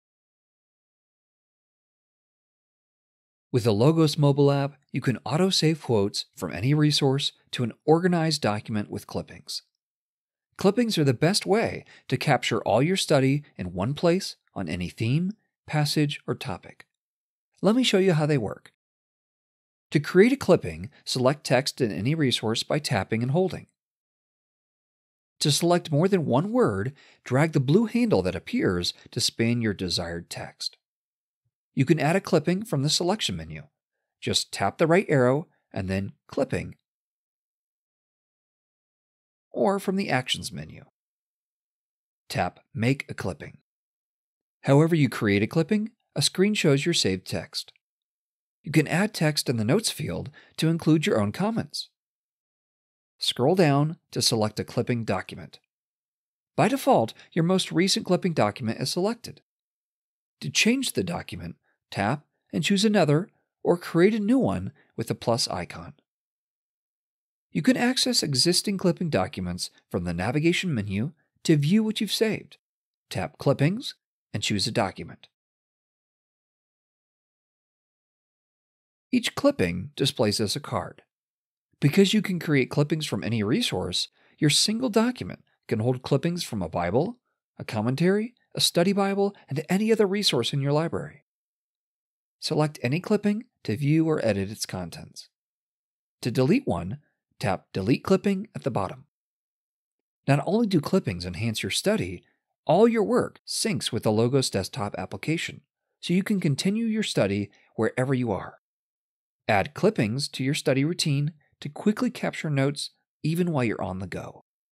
Mobile Clippings | Logos Bible Software Support
Clippings are custom collections of saved text from resources in your library. They help you pull together important quotes and insights on any theme, passage, or topic.